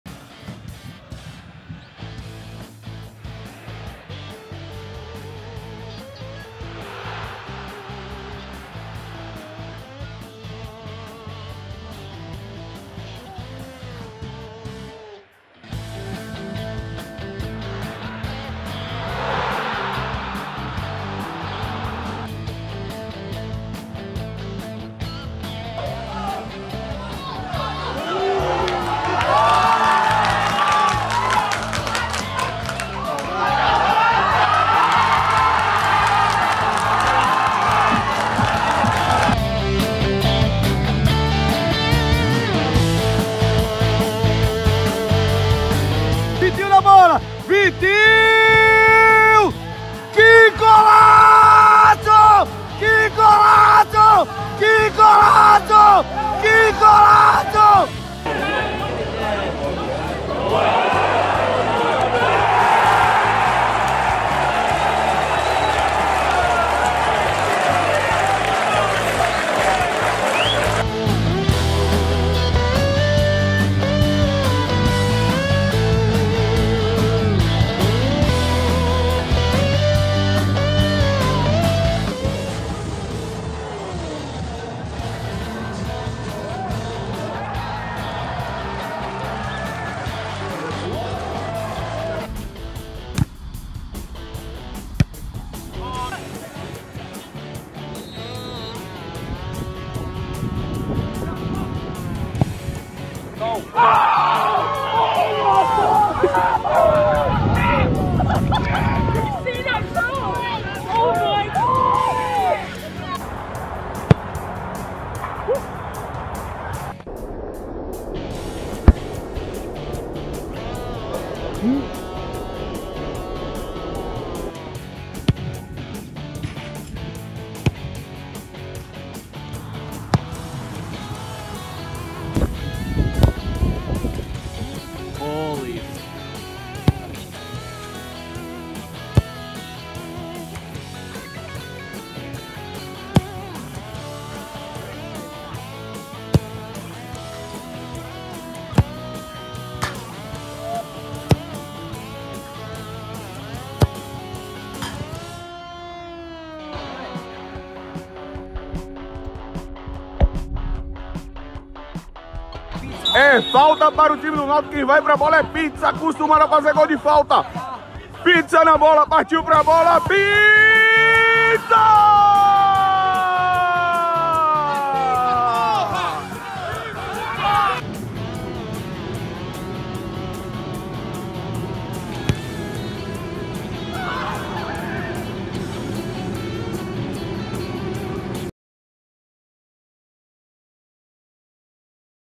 This Strike From More Than 60 Meters Deserves The Puskás Award!
This absolute worldie comes to us straight from Argentina! Mateo Coronel, the Atletico Tucumán midfielder, scored from an incredible strike from more than 60 minutes out. A true treat for the eyes!

It's good to hear all kinds of reactions, we'll definitely use your feedback for our next videos.